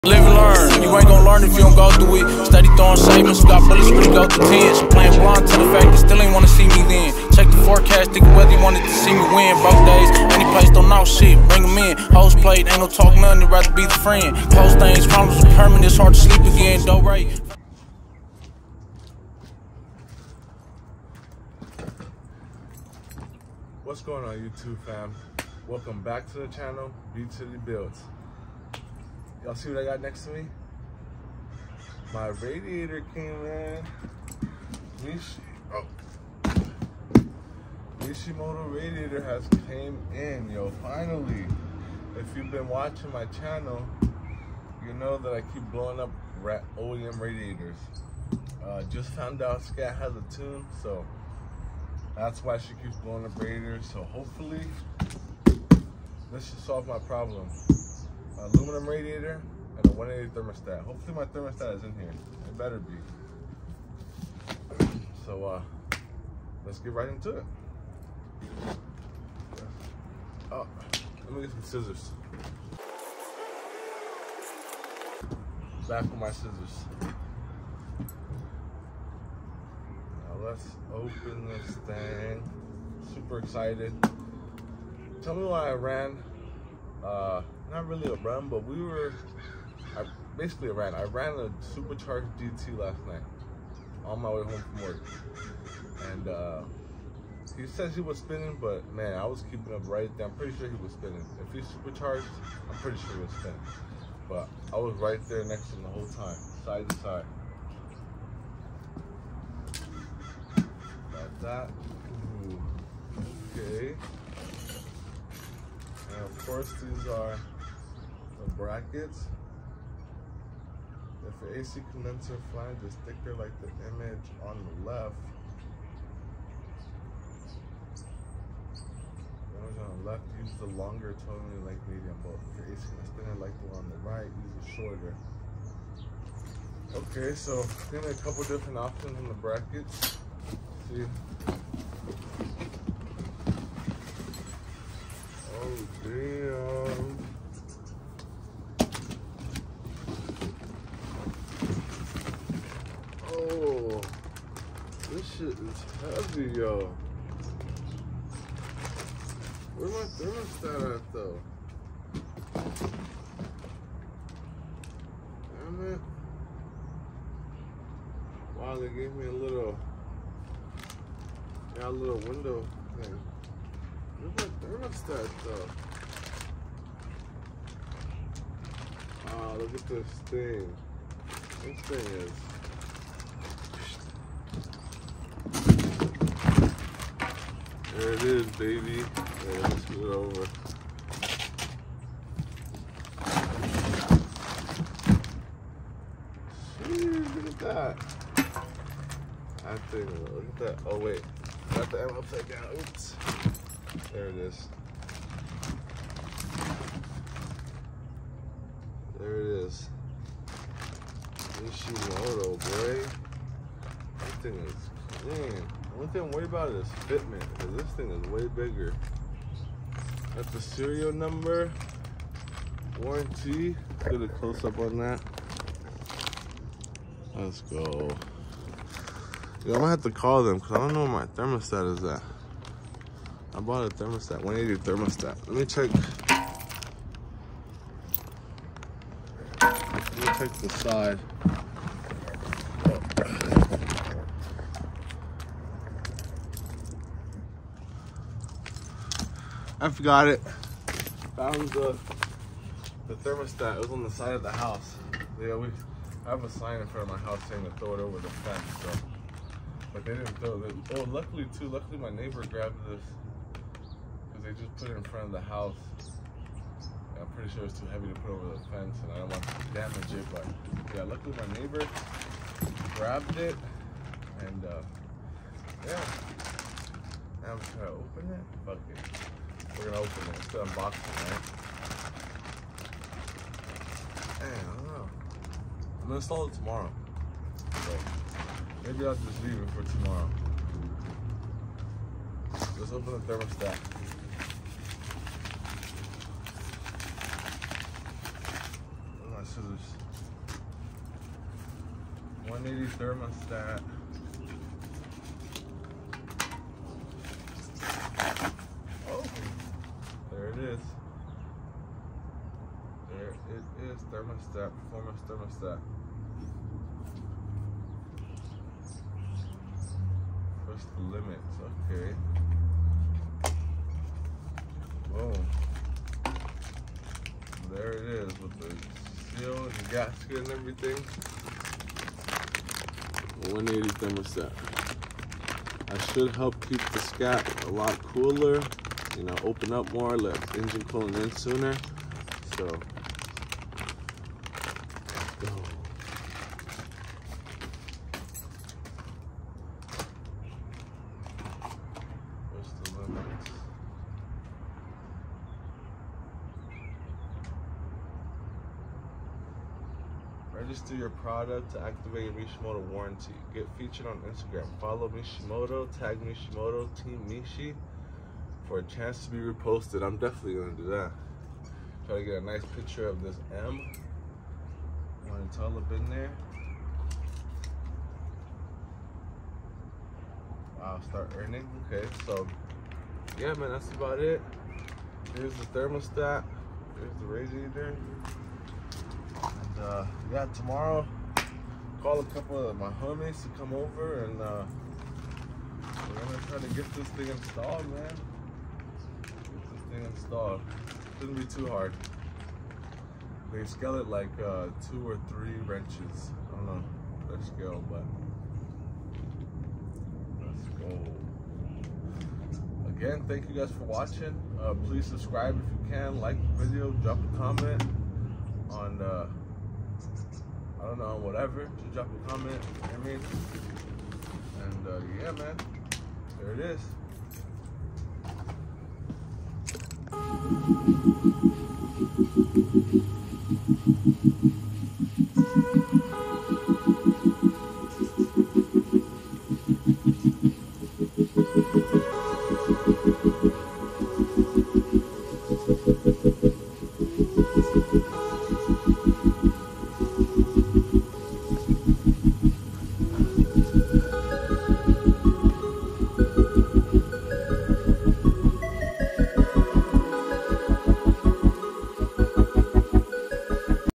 Unboxing @mishimoto radiator!
Unboxing my new rad! I already installed it, once I finish editing the video, it will be posted! Follow the build!